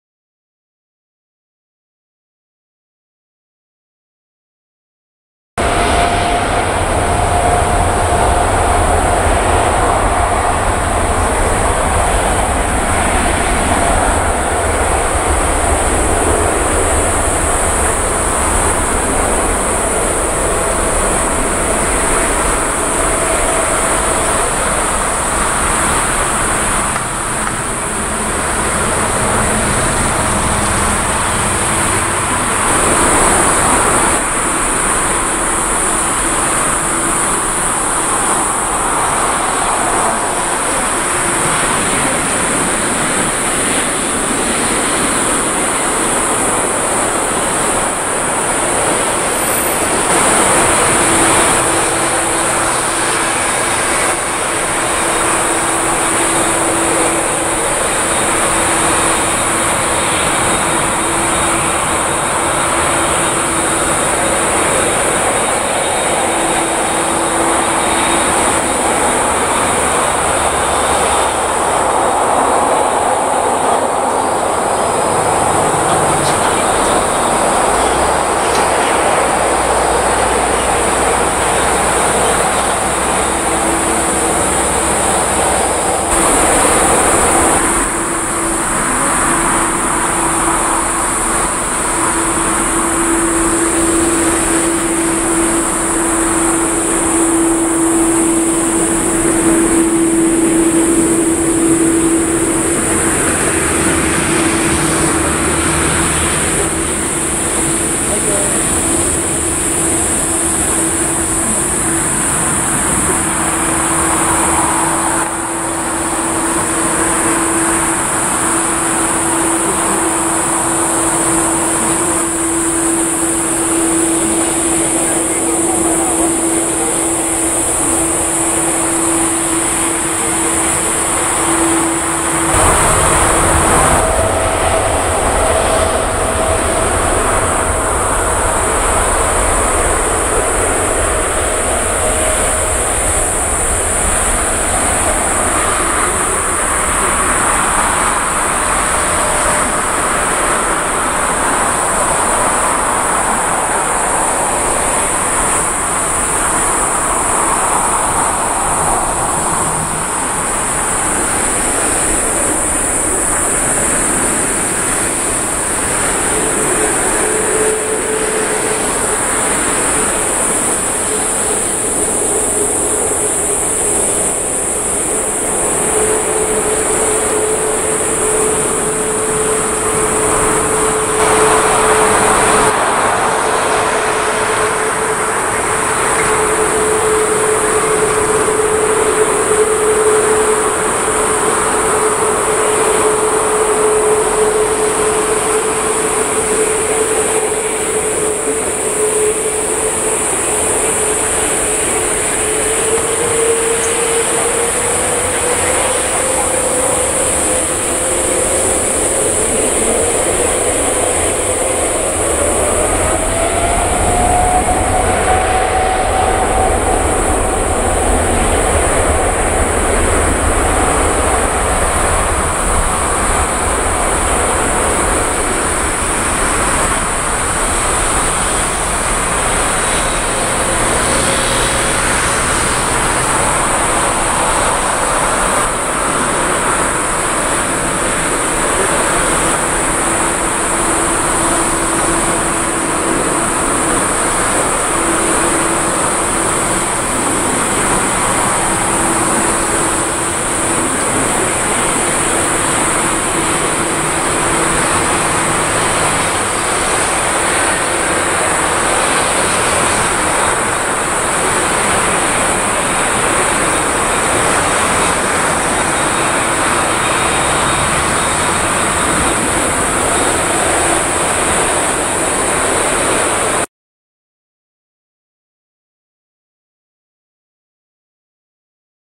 ✈✈Takeoff at night 最新機種で就航 NH801便NRT/SIN (All Nippon Airways)Boeing 787-10 JA900A Narita RWY16R成田空港
成田／シンガポール線はANAに2機だけのBoeing787-10が就航しております。
Boeing 787-10
JA900A